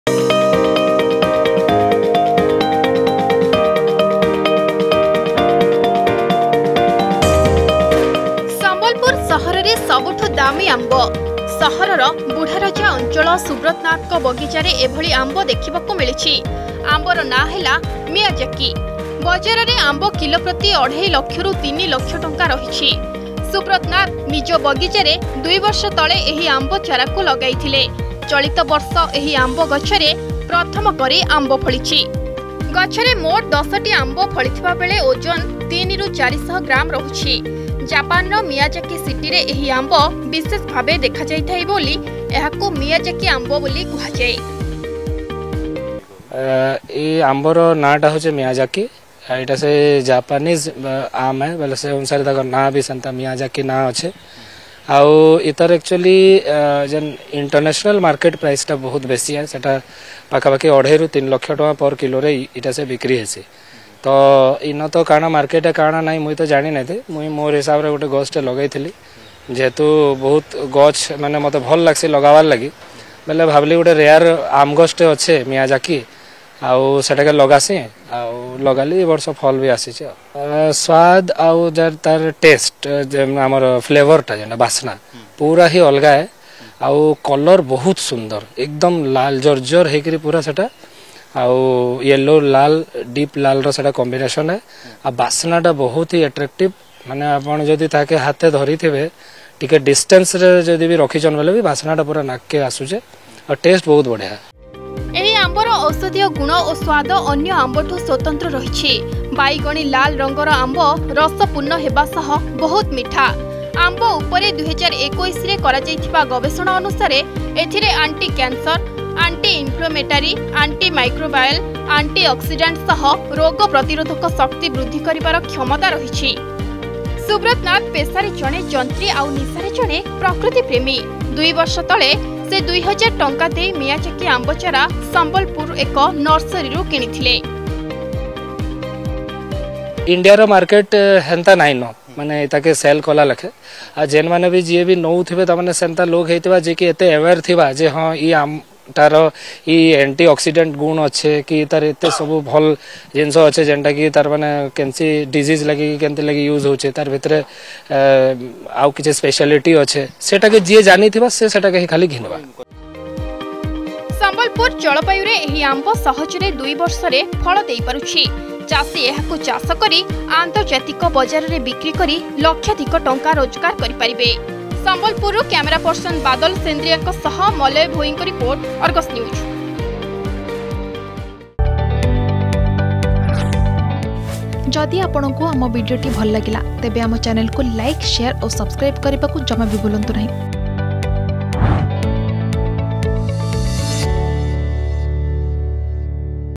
ସମ୍ବଲପୁର ସହରରେ ସବୁଠୁ ଦାମୀ ଆମ୍ବ || Expensive Mango || Sambalpur
ସମ୍ବଲପୁରରେ ଲକ୍ଷାଧିକ ଟଙ୍କିଆ ଆମ୍ବ ଫଳୁଛି । ଏହି ଆମ୍ବ ସବୁଠୁ ଦାମୀ ଆମ୍ବ ବୋଲି ଜଣାପଡିଛି  । ମିୟାଜାକି ନାମର ଆମ୍ବ ଯାହାର ମୂଲ୍ୟ କିଲୋପ୍ରତି ୩ ଲକ୍ଷ । ସହର ଜଣେ ଯୁବ ପ୍ରକୃତି ପ୍ରେମୀ ଏଭଲି ଆମ୍ବ ଚାଷ କରି ଚର୍ଚ୍ଚାରେ ଅଛନ୍ତି ।